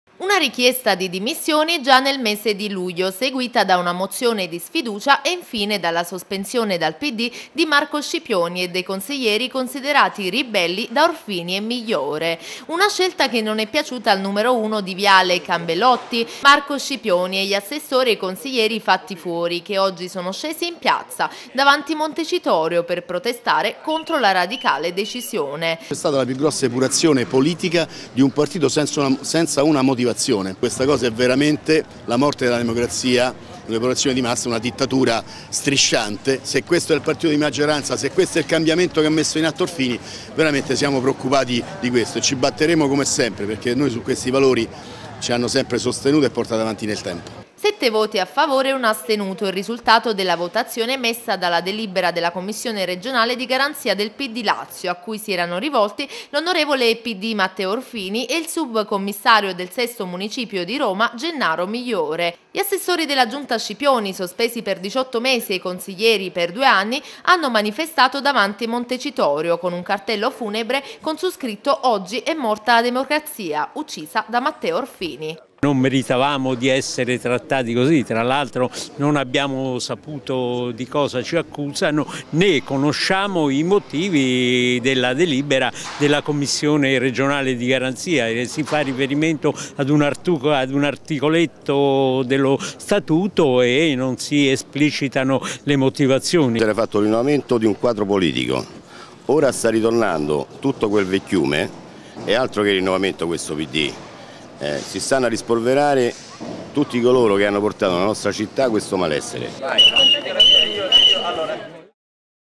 RETESOLE TG LAZIO Montecitorio, protestano degli “epurati” dal Pd
Una richiesta di dimissioni già nel mese di luglio seguita da una mozione di sfiducia e infine dalla sospensione dal Pd di Marco Scipioni e i dei consiglieri considerati "ribelli" da Orfini e Migliore. Una scelta che non è piaciuta al numero uno di viale Cambellotti, Marco Scipioni, e agli assessori e i consiglieri fatti fuori che oggi sono scesi in piazza, davanti Montecitorio per protestare contro la radicale decisione,.
7 voti a favore e 1 astenuto il risultato della votazione emessa dalla delibera della Commissione Regionale di garanzia del Pd lazio a cui si erano rivolti l'Onorevole Pd Matteo Orfini e il Subcommissario del sesto municipio di roma Gennaro Migliore.
Gli assessori della giunta Scipioni, sospesi per 18 mesi, e i consiglieri per due anni, hanno manifestato davanti Montecitorio con Un cartello funebre con su scritto:«Oggi è morta la democrazia uccisa da Matteo Orfini».
Cristina Autore